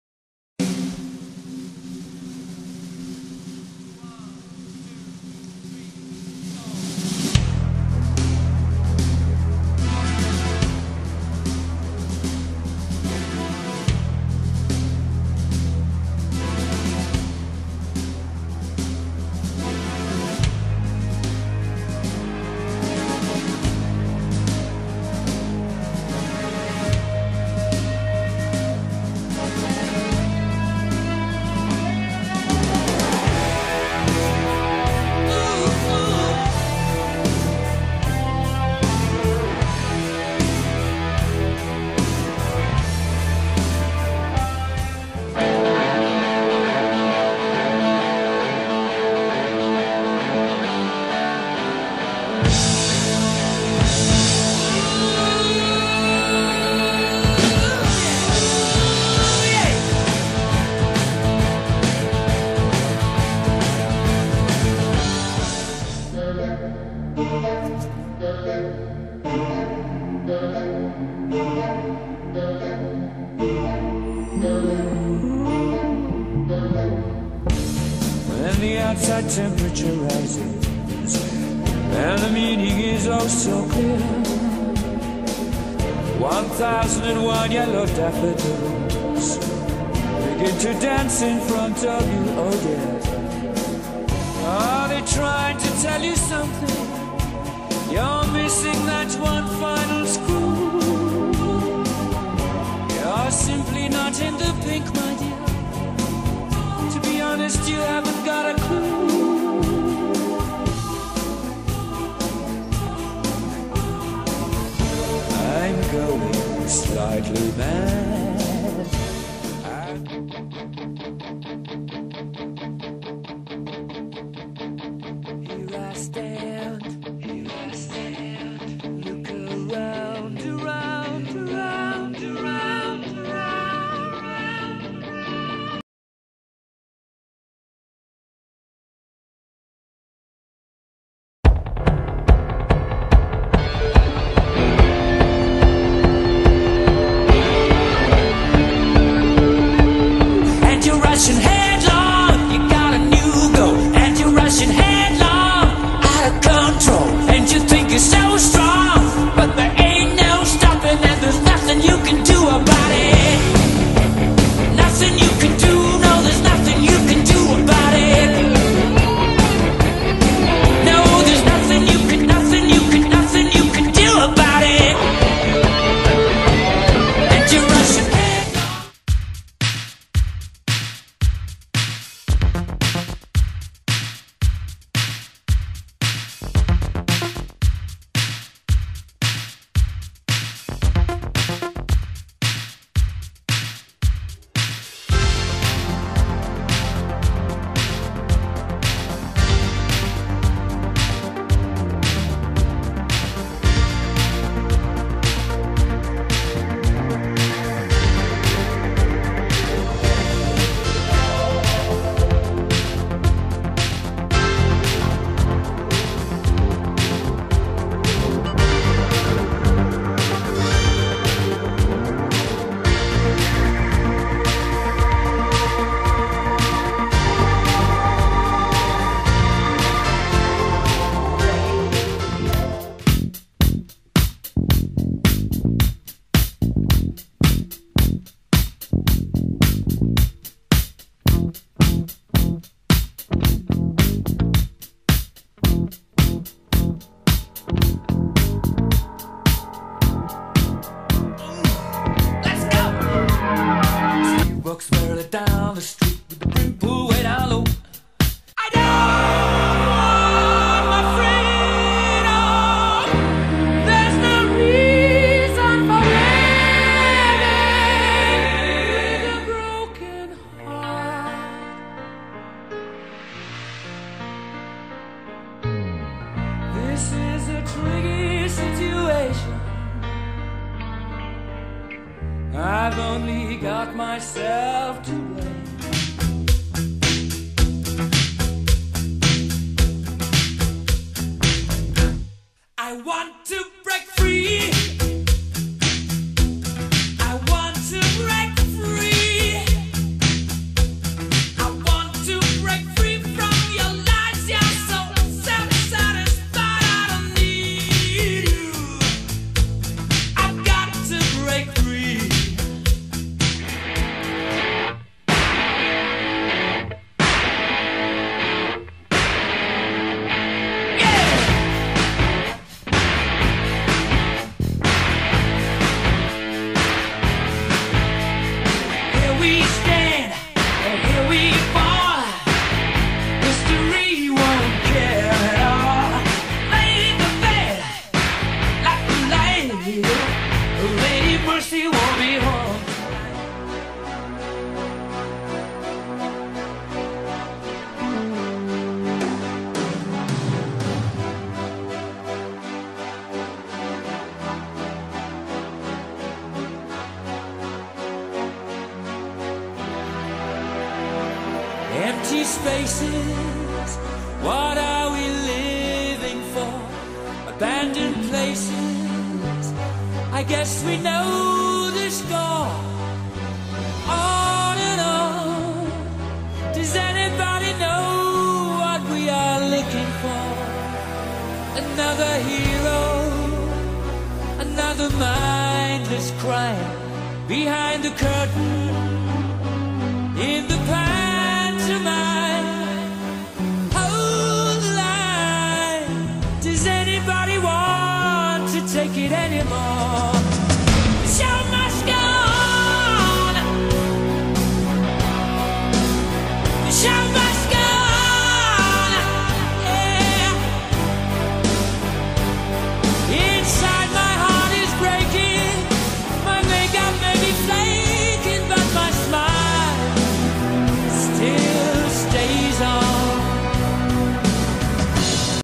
Queen Medley
My favourite Queen songs mixed in a medley with pics